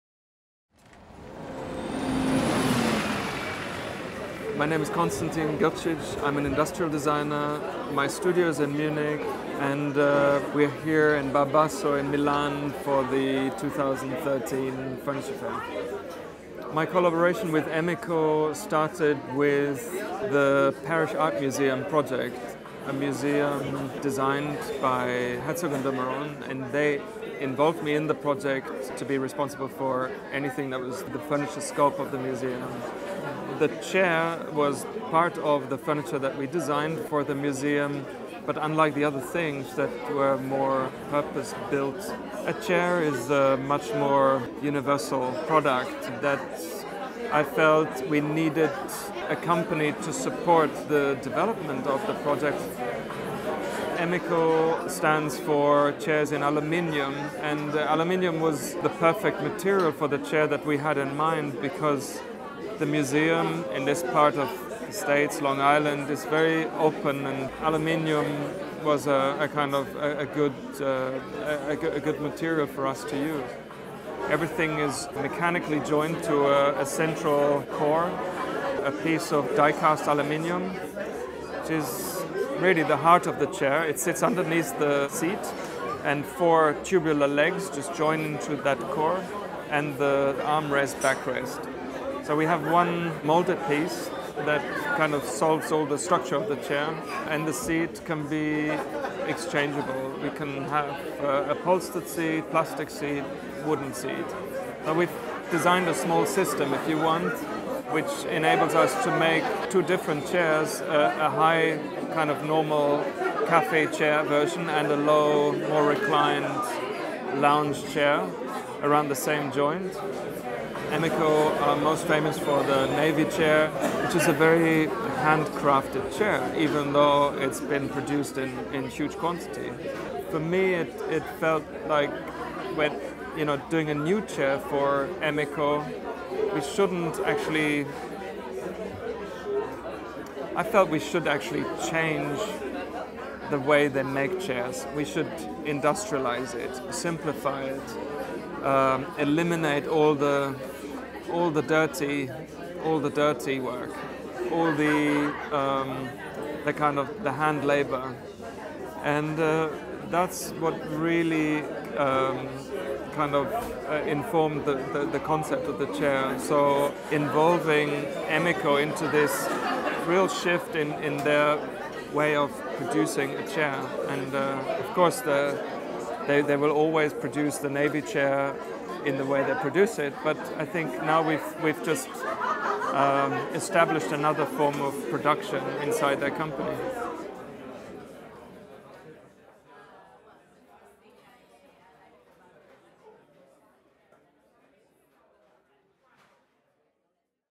"I felt we should change the way Emeco makes chairs" - Konstantin Grcic
Designer Konstantin Grcic tells Dezeen that American furniture company Emeco had to industrialise its production methods to produce his new Parrish chair in this video interview filmed in Milan.

Grcic originally designed the Parrish chair as part of a range of furniture for Herzog & de Meuron's barn-like Parrish Art Museum on Long Island, completed in 2012.

The chair was launched by American furniture company Emeco as a commercial product at Milan earlier this year.